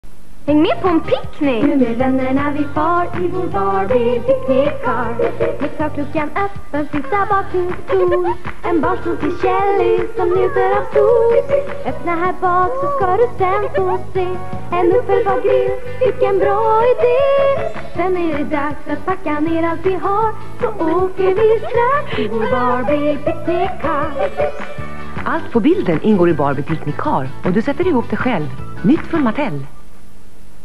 Barbie Reklam (1995-1996) - Barbie Picnic Car
Här kommer en reklam ifrån antingen 1995 eller 1996 för Barbie's Picnic Car.

Om ändå en riktig bil kunde göra allt detta. Hehe.

En vän till mig hittade den här reklamen på ett gammalt band med "Swat Kats".